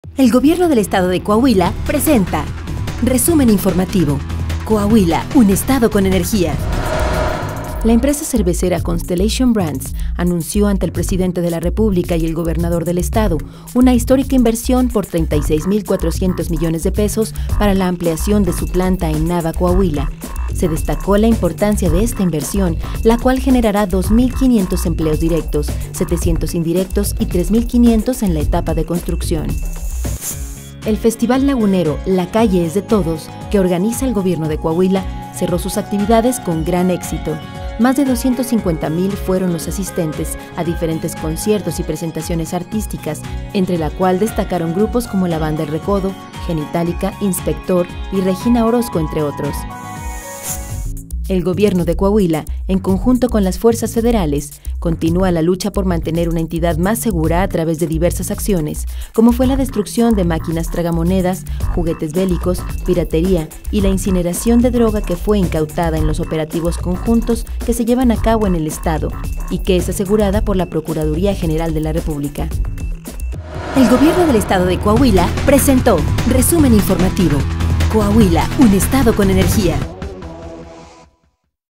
Resumen Informativo Laguna con Energía 22 al 28 de junio 2015
Resumen Semanal de noticias con las acciones más importantes realizadas en la Región Laguna por el Gobierno del Estado de Coahuila.